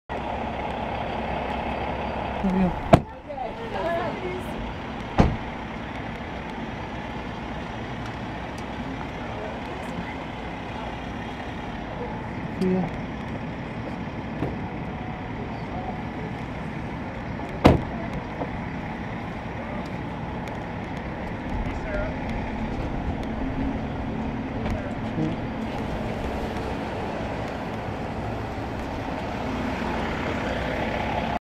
Sophia Bush and James Lafferty depart the Global Green US...
Sophia Bush and James Lafferty depart the Global Green USA's 10th Annual Pre-Oscar Party in Hollywood, 02/20/13